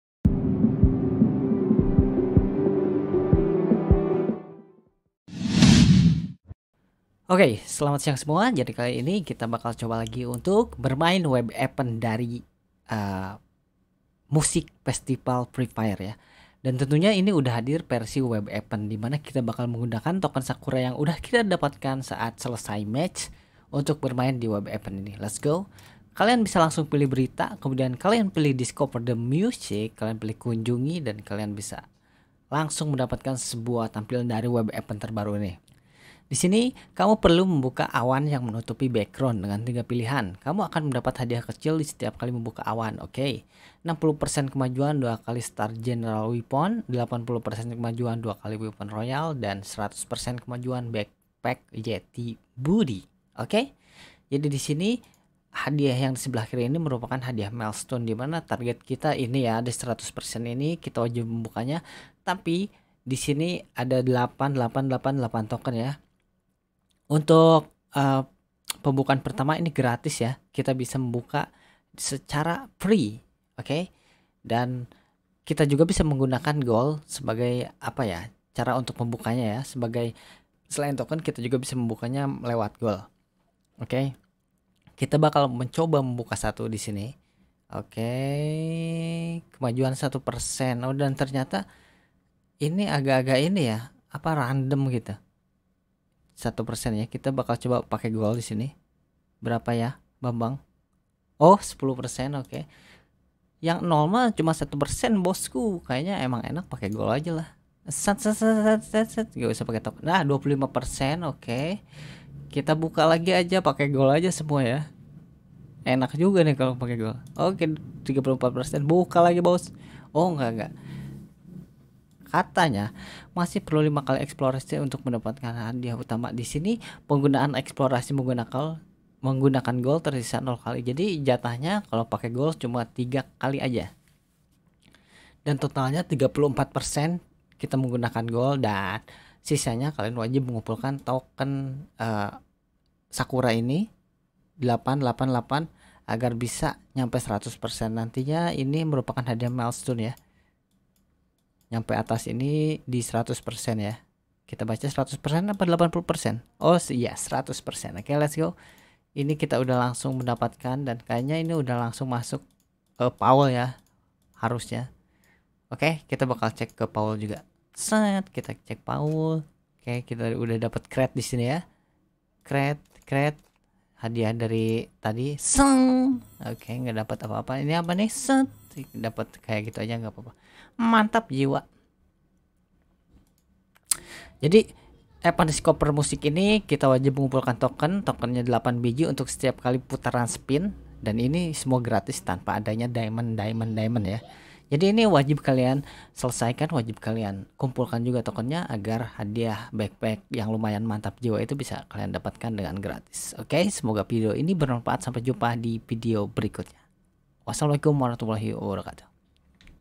CARA BERMAIN WEB EVENT DISCOVER MUSIC FREE FIRE
Token sakura yang sudah kita dapatkan tadi mulai hari ini sudah bisa di gunakan untuk spin di web event discover music. Nah Bagaimana sih cara bermain untuk web event discover musik ini ?